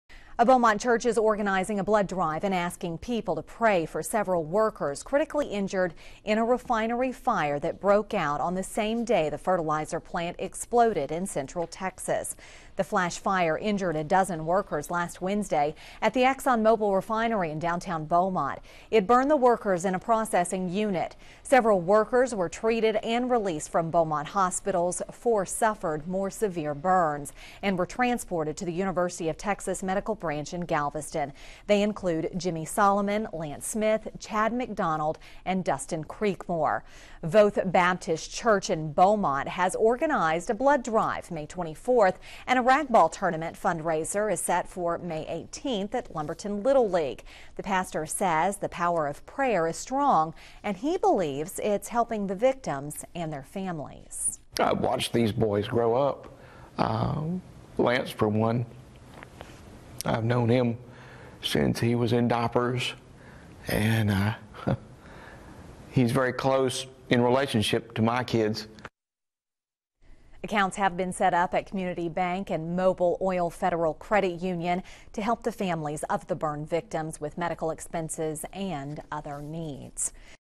Church works to help burn victims
A Beaumont church is organizing a blood drive and asking people to pray for several workers critically injured in a refinery fire that broke out on the same day the fertilizer plant exploded in Central Texas.The flash fire injured a dozen workers last Wednesday at the Exxon Mobil refinery in downtown Beaumont.It burned the workers in a processing unit.  Several workers were treated and released from Beaumont hospitals. Four suffered more severe burns and were transported to the University of Texas medical branch in Galveston.They include Jimmie Solomon, Lance Smith, Chad McDonald, and Dustin Creekmore.Voth Baptist Church in Beaumont has organized a blood drive May 24th and a rag ball tournament fundraiser is set for May18th at Lumberton Little League.The pastor says the power of prayer is strong and he believes it's helping the victims and their families.